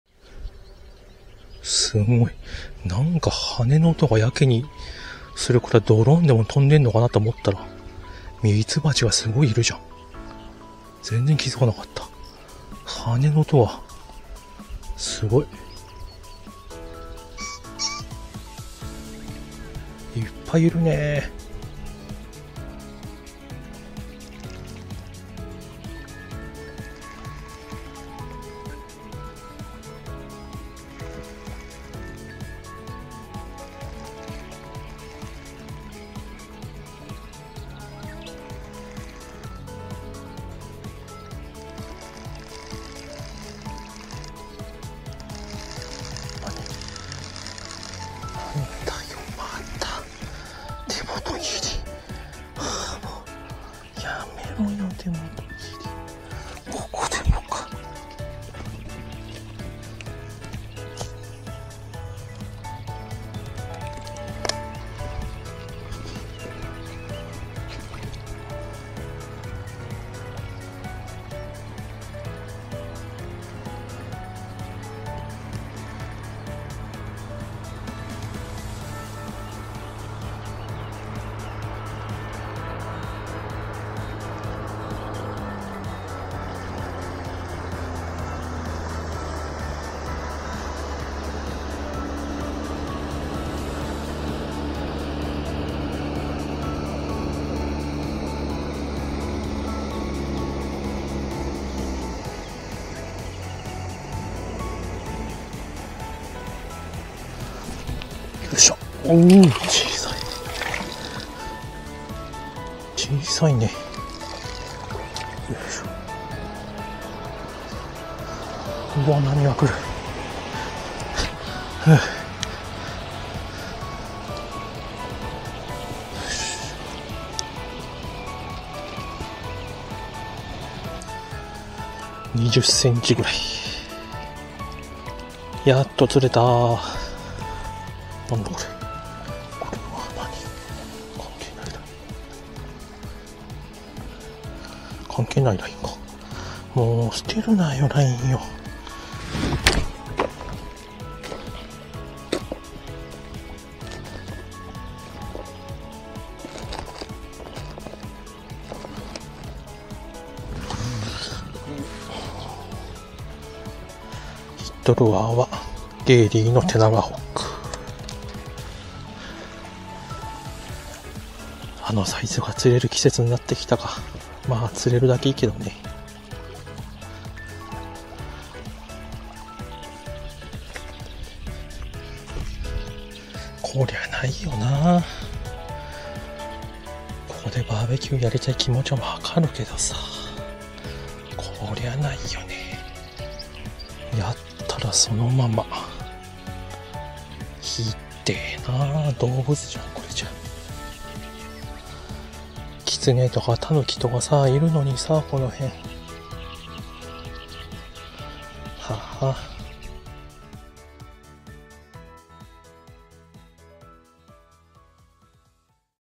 陸の孤島 川スモールマウスバス テナガホッグ！
水温も上がってきて小型スモールも釣れるようになってきましたね！
ゲーリーヤマモトのルアーだけで釣りしています！